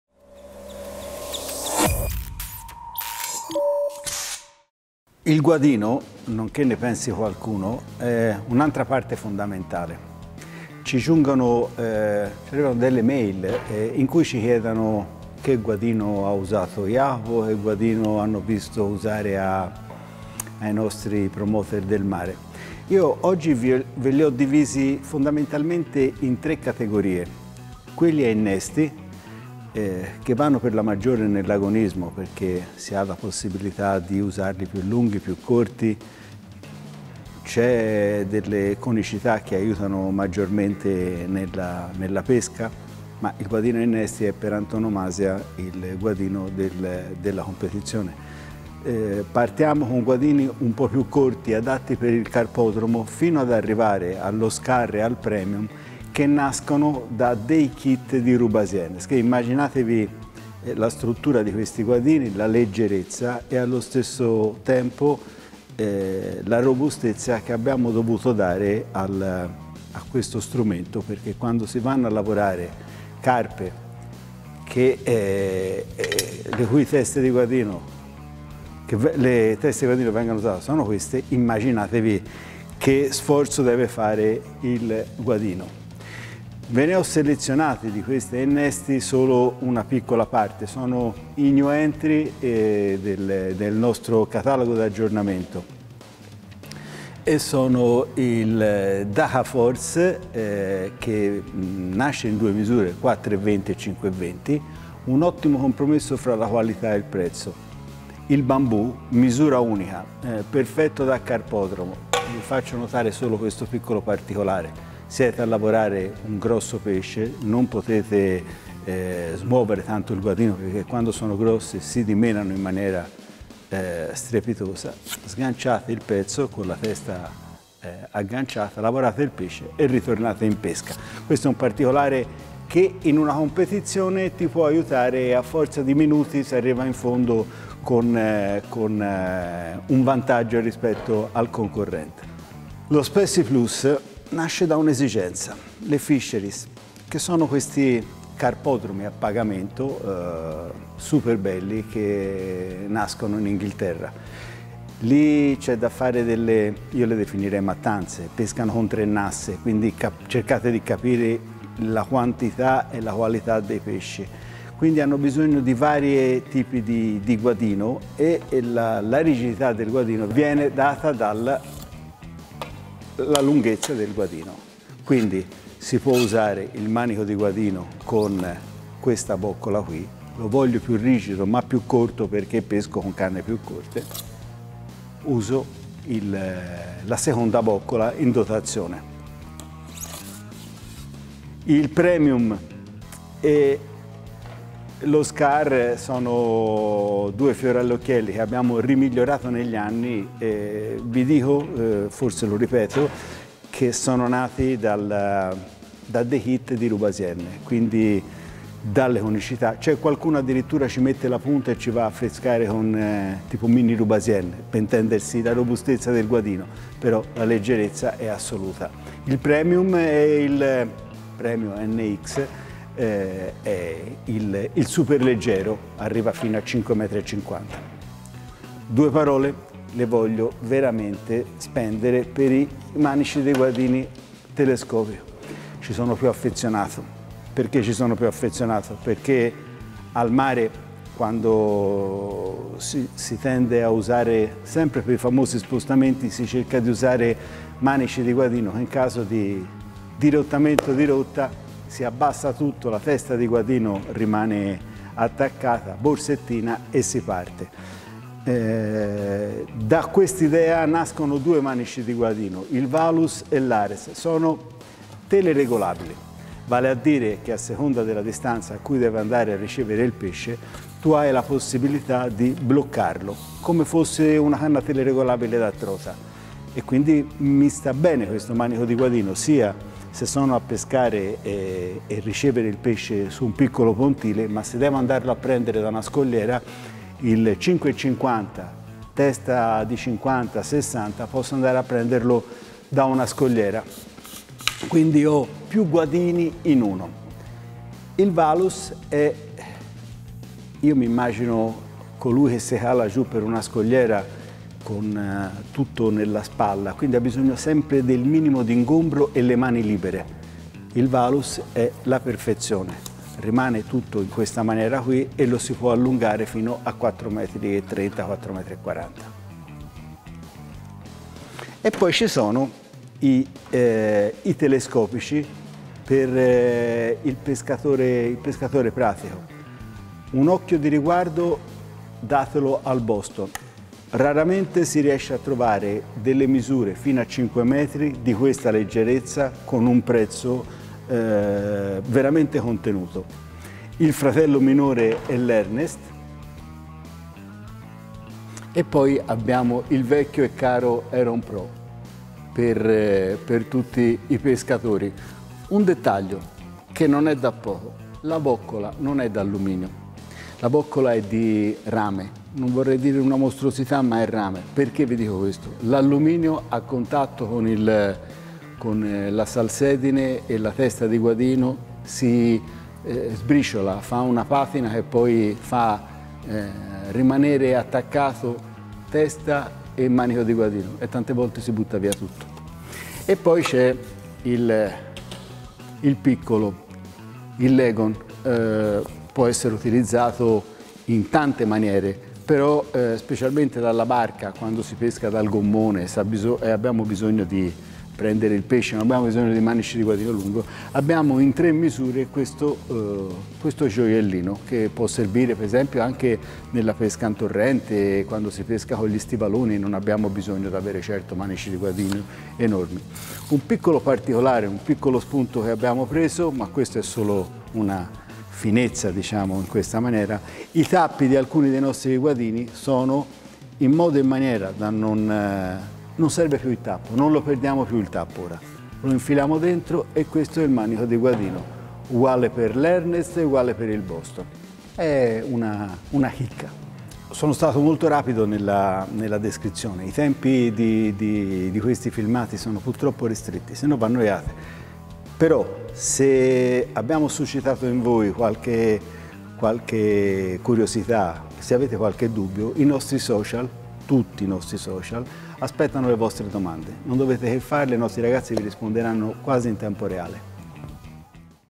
COME SCEGLIERE IL MANICO DI GUADINO
In questo video Gianluca Meniconi ci spiega le caratteristiche per scegliere al meglio il nostro manico di guadino.

Varie tipologie e misure per ogni esigenza del pescatore.